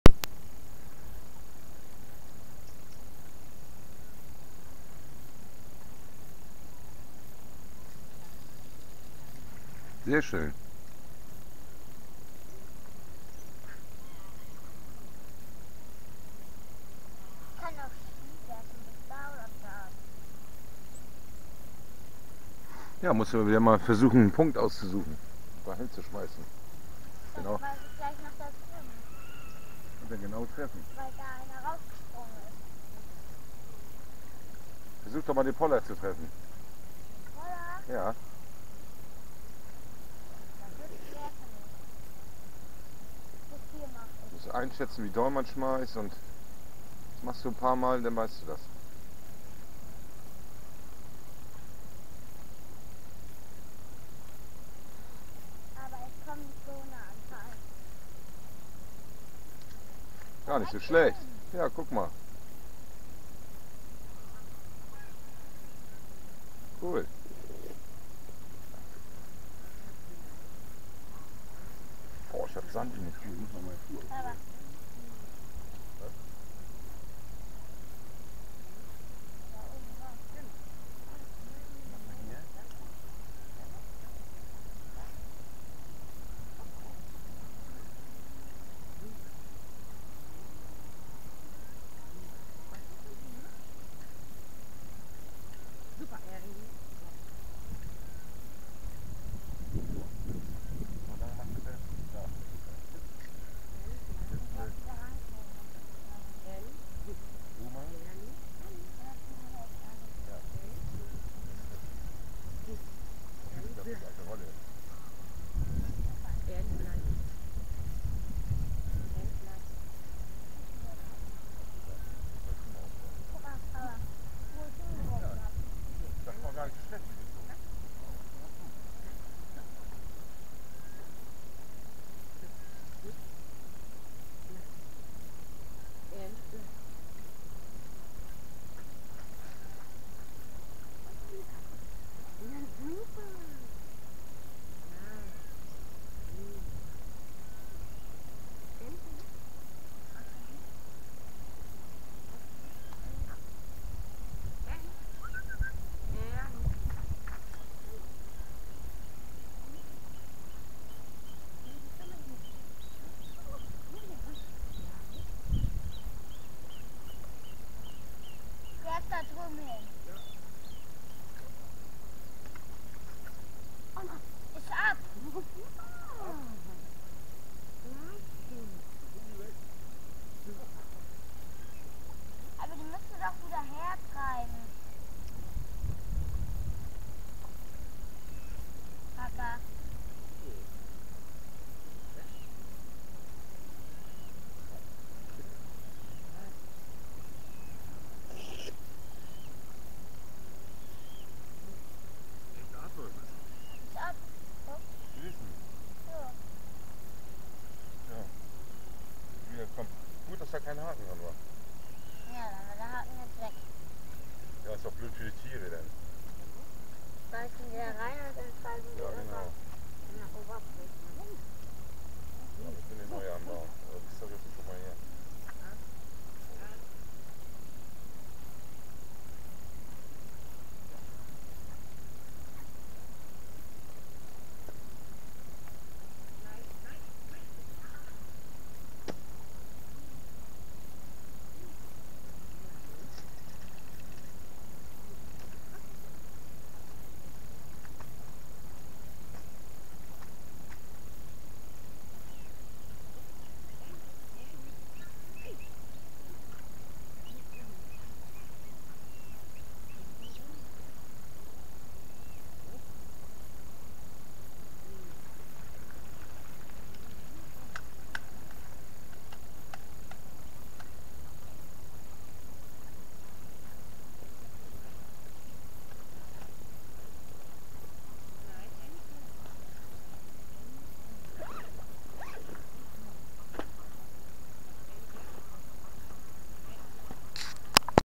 Erste Wurfübung mit der Angel.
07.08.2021  Mein Sohn macht seine ersten Wurfübungen mit der Angel. Hornhecht und Hering hat er schon gefangen, aber ich musste immer auswerfen. Er möchte jetzt selbst auswerfen. Hat er gut gemacht!